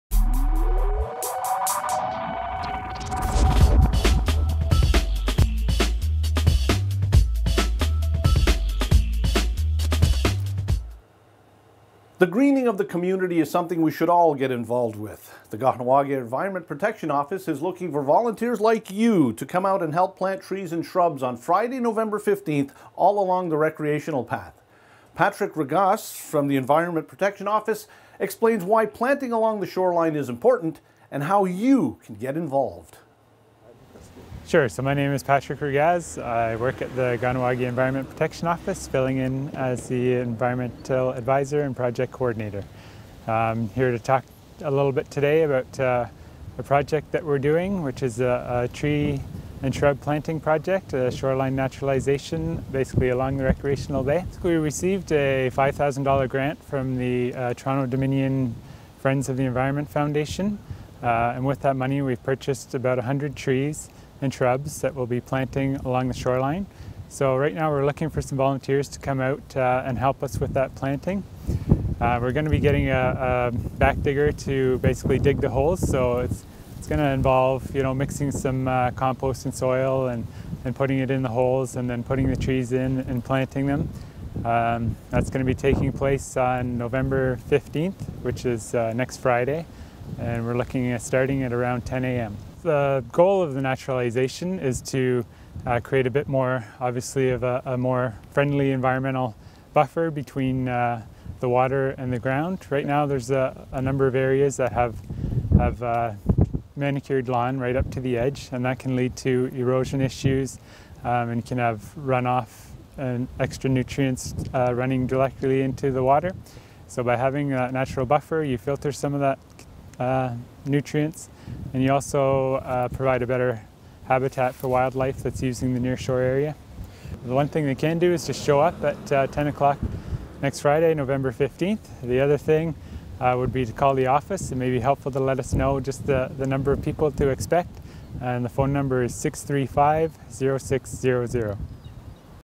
Planting to protect our Shoreline
The greening of the community is something we should  all be involved with. The Kahnawà:ke Environment Protection Office is looking for volunteers to come out and help plant trees and shrubs Friday November 15th along the recreational path.  Patrick Ragaz from Environment explains why planting along the shoreline is important and how you can get involved.